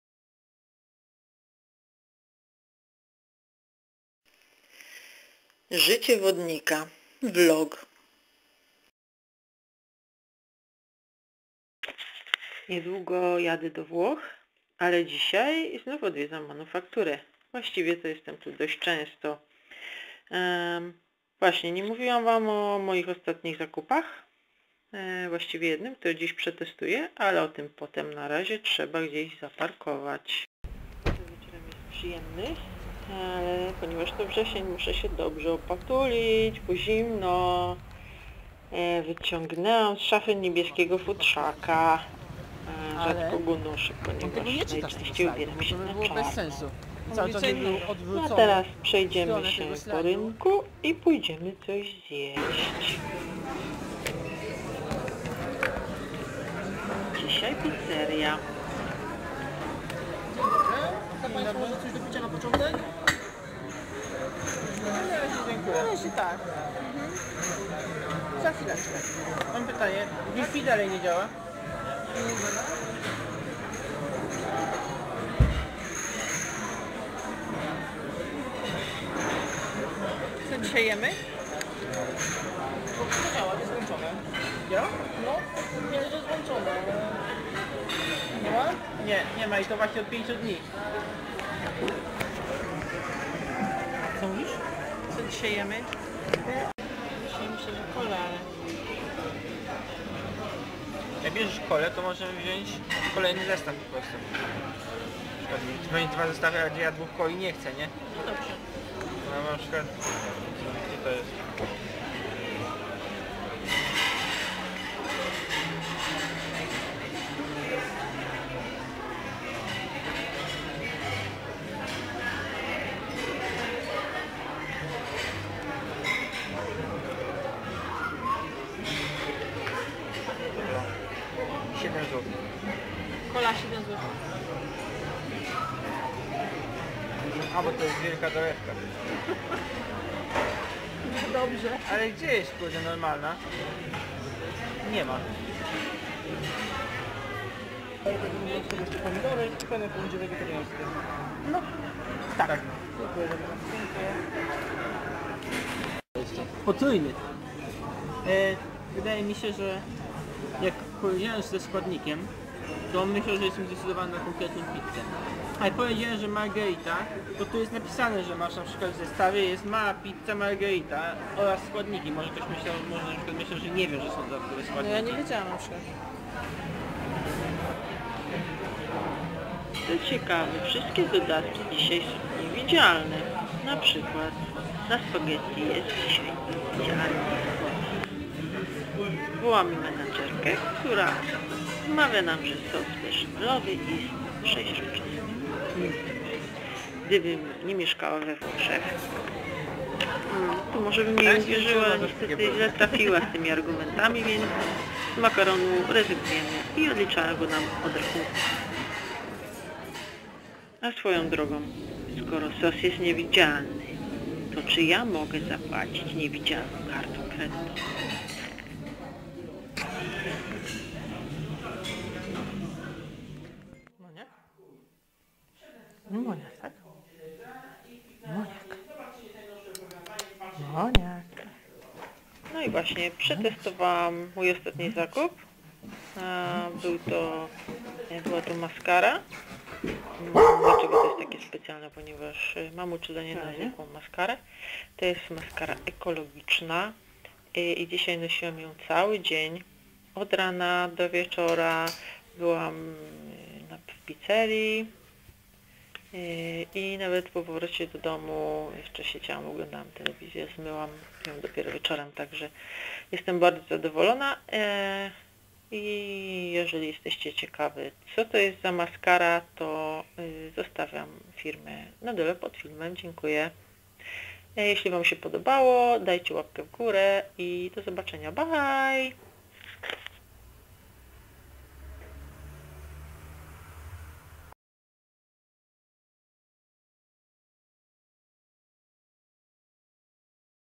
Vlog nr.2 Życie WODNIKA : Kolacja w Manufakturze
VLOG - DRUGI odcinek z serii "Życie Wodnika". Jest to seria blogów dokumentujących moje życie w Polsce i we Włoszech.
W tym odcinku jedziemy na kolację do Manufaktury w Łodzi. Za parę dni będę już we Włoszech...

Mascara, o której mówiłam jest z firmy LAVERA (black)

I make also songs so you are welcome to watch my videos in my Ergaya song's playlist.

Kisses Ergaya